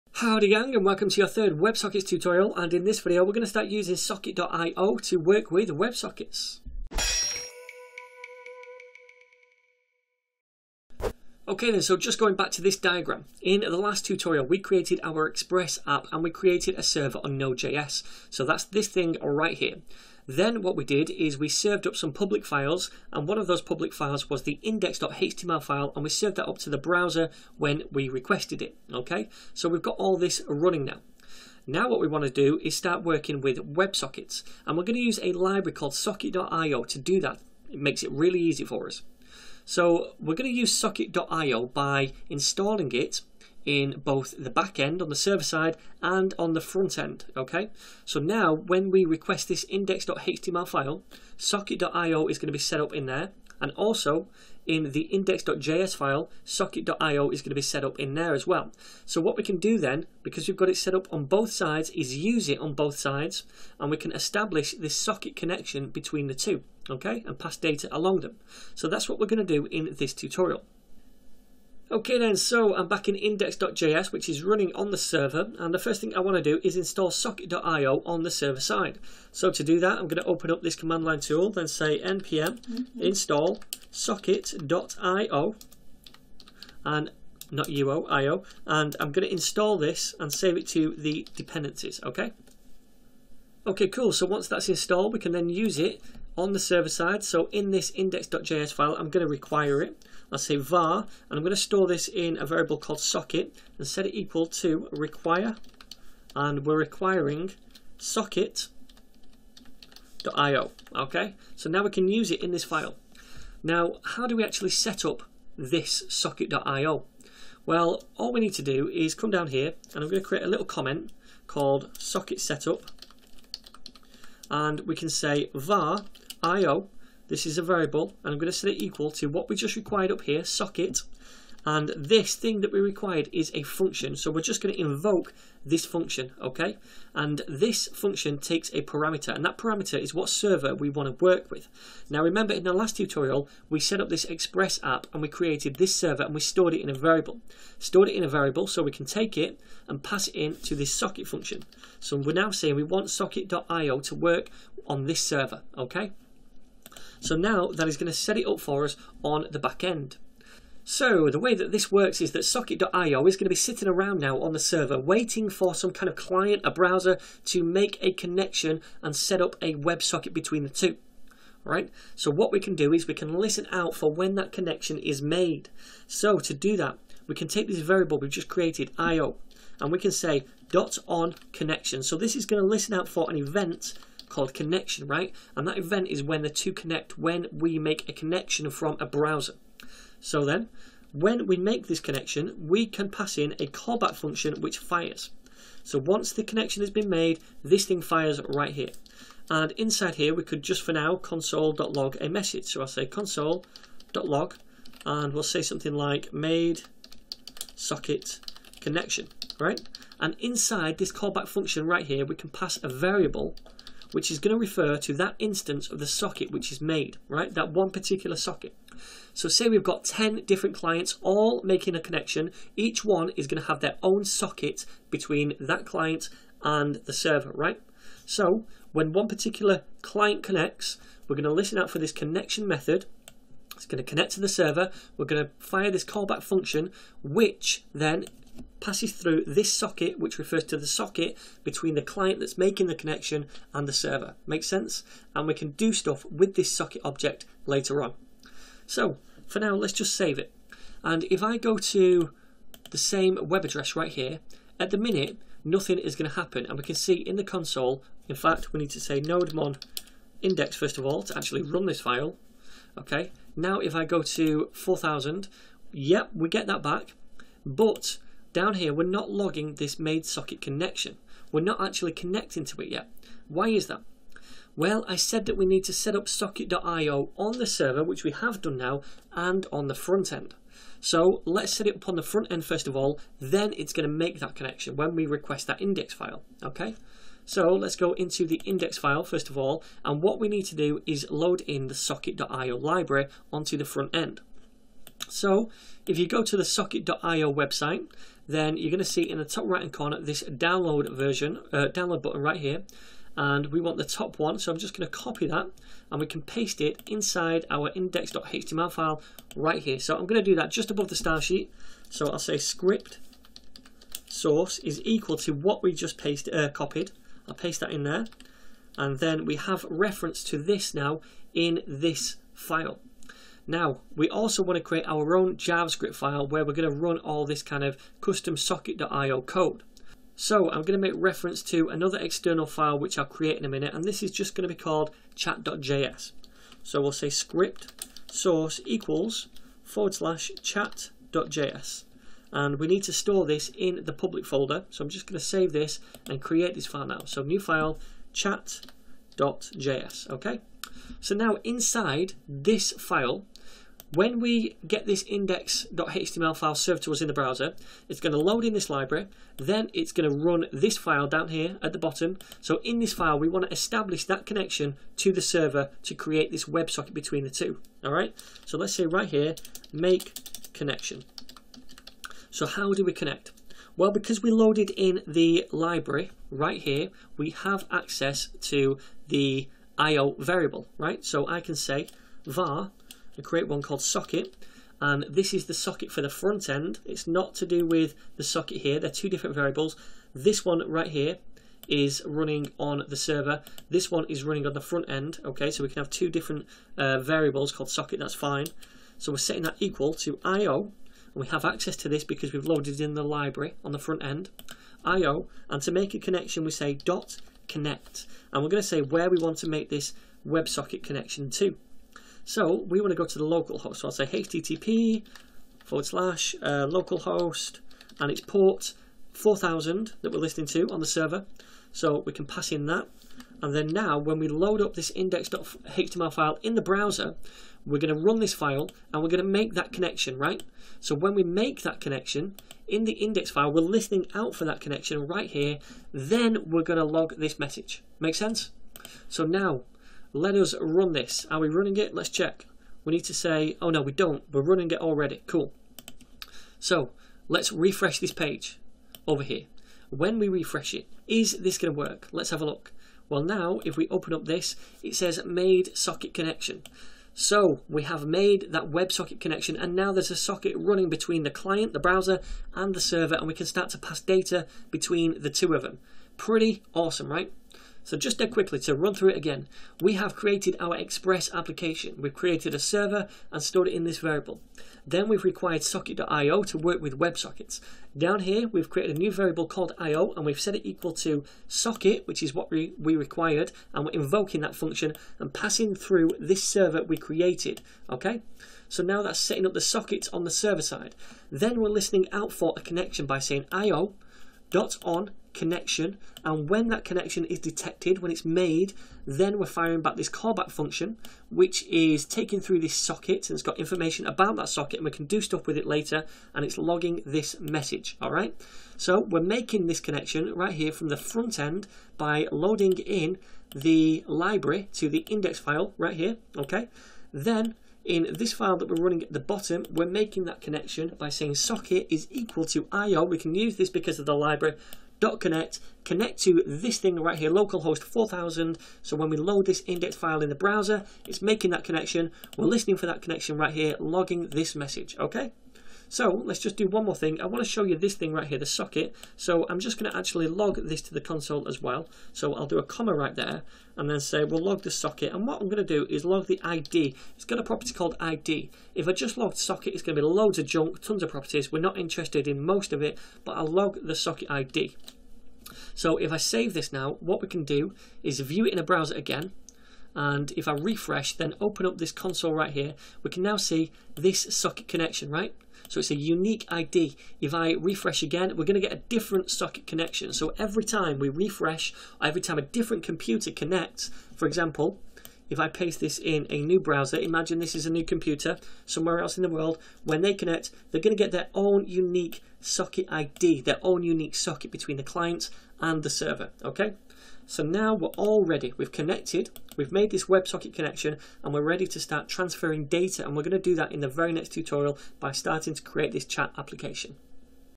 WebSockets (using Socket.io) Tutorial #3 - Using Socket.io
Hey gang, in this WebSockets tutorial I'll show you how to use socket.io to establish a connection from the client to the server. Once that's out of the way, we can start to build our live chat app :).

Social Links